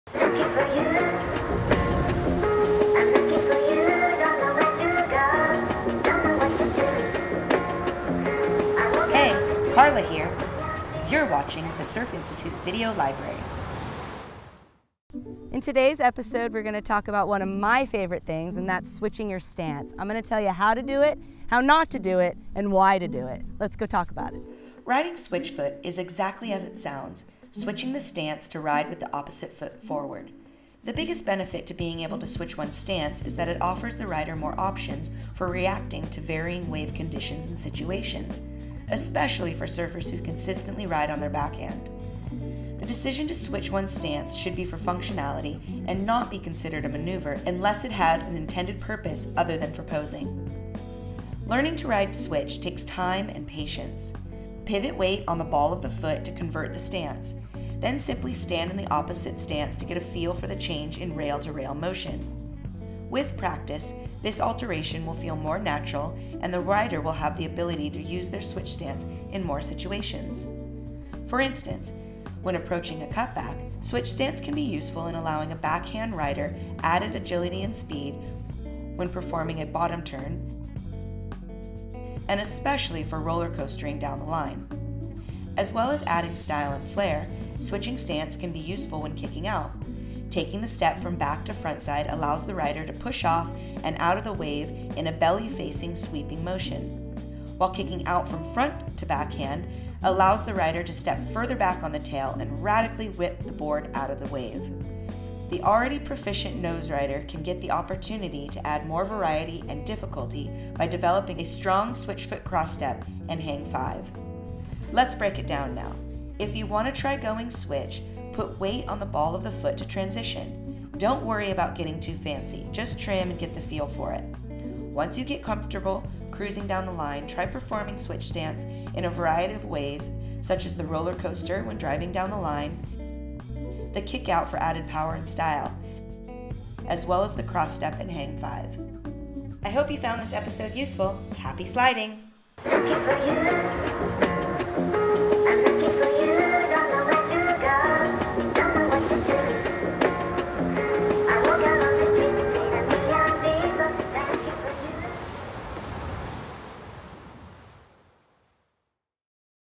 The Surf Institute - Episode 19 - Switchstance
Riding switchfoot is exactly as it sounds: switching the stance to ride with the opposite foot forward.